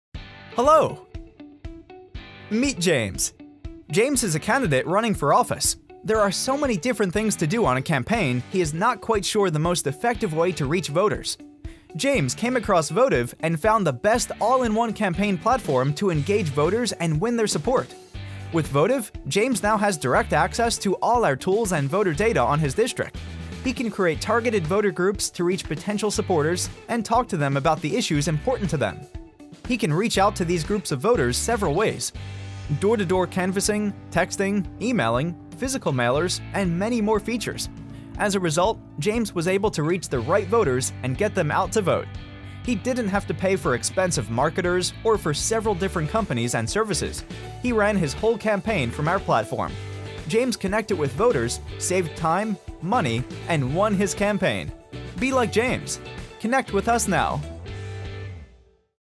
Vottiv Political Campaign and Services Explainer Video
Meet James, a political candidate. Find out how Vottiv political campaign platform helped him connect with voters, and win his campaign.

Vottiv offers software and high-quality data to political campaigns of all sizes to make connecting with voters simple, easy, and affordable for anyone.